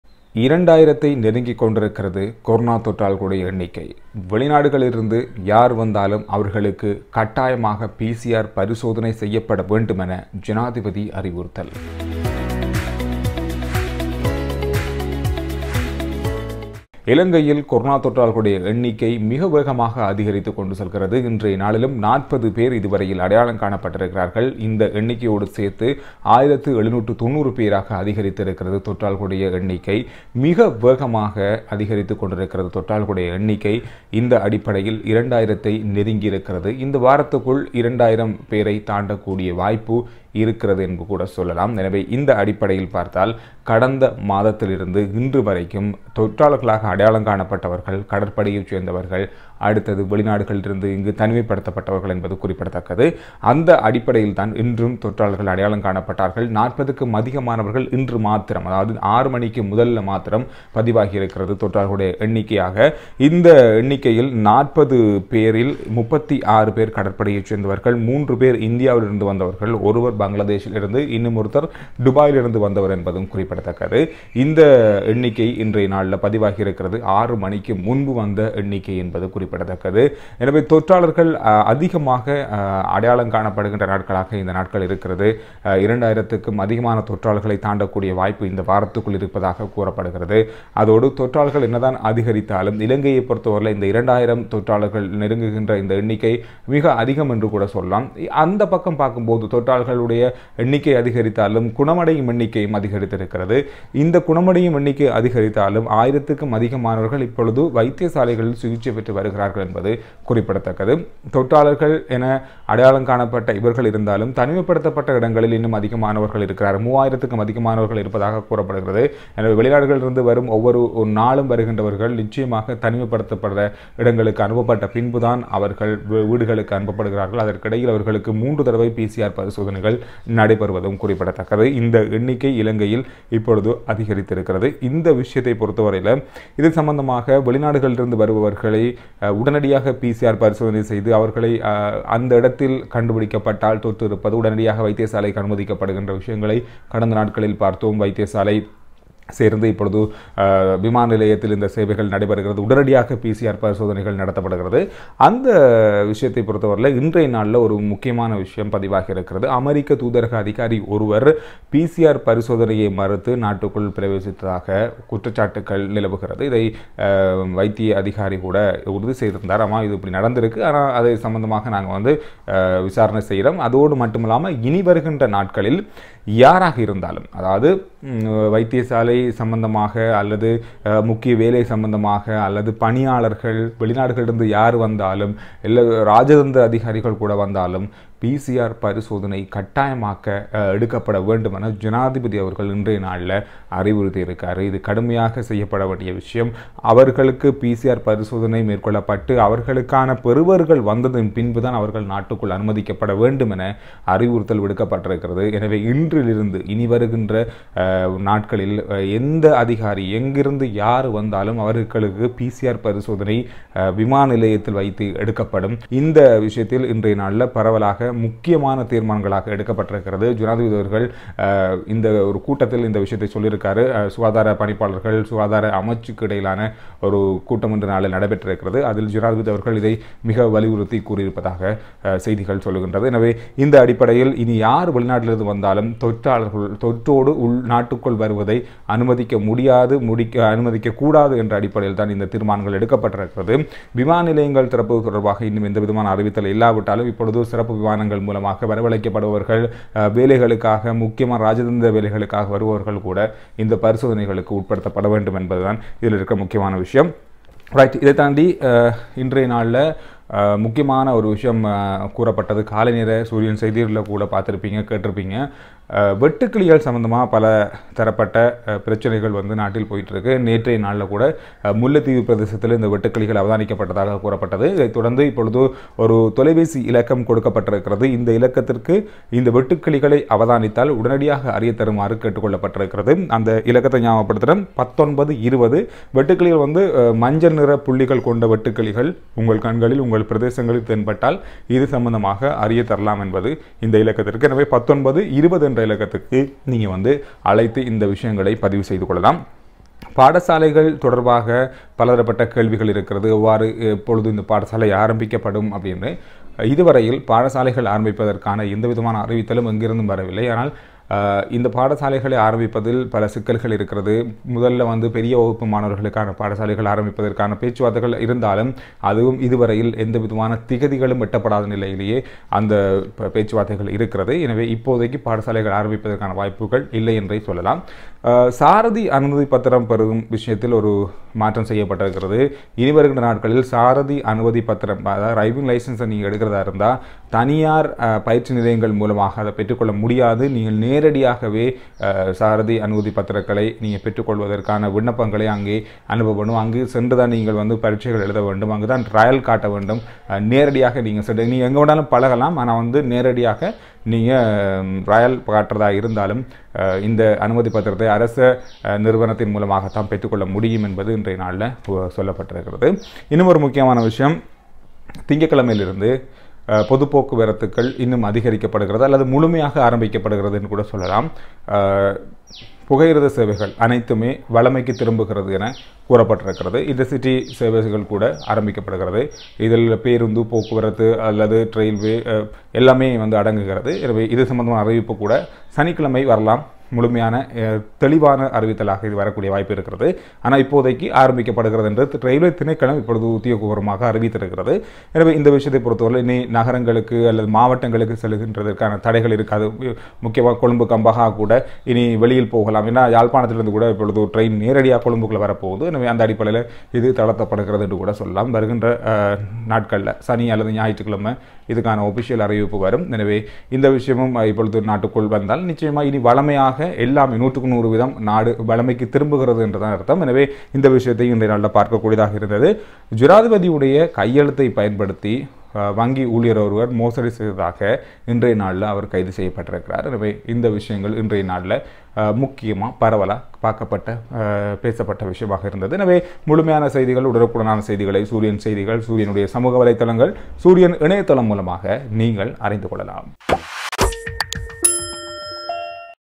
PCR பரிசோதனையை மறுத்த அதிகாரி | Sri Lanka + Corona News | Sooriyan Fm | Rj Chandru
2,000 பேரை நெருங்கும் கொரோனா  | Sri Lanka + Corona News | Sooriyan Fm | Rj Chandru
பரிசோதனையை மறுத்த அதிகாரி

Sri Lanka’s Covid-19 cases surge to 1,790 with 40 new detections

Even though the number of COVID-19 cases reported within the Navy are relatively high, all the cases are being reported from quarantine centres, the Navy said today.

இலங்கையில் COVID19 தொற்றாளர்களின் எண்ணிக்கை 1,790 ஆக உயர்வு

தனியார் நிறுவனங்கள் நடத்தும் சாரதி அனுமதிபத்திர சோதனைகளை ரத்து செய்வதற்கான போக்குவரத்து அமைச்சர் மஹிந்த அமரவீரவின் முன்மொழிவுக்கு அமைச்சரவை ஒப்புதல் அளித்துள்ளது.

விவசாயிகள், தங்கள் பகுதிகளில் மஞ்சள் புள்ளிகள் கொண்ட வெட்டுக்கிளிகளை அவதானித்தால் 1920 என்ற இலக்கத்திக்கு அறிவுக்கவும்.

Coronavirus disease spreads primarily through contact with an infected person when they cough or sneeze. It also spreads when a person touches a surface or object that has the virus on it, then touches their eyes, nose, or mouth.
corona virus tamil latest news

corona sri lanka news todaycovid 19 news tamil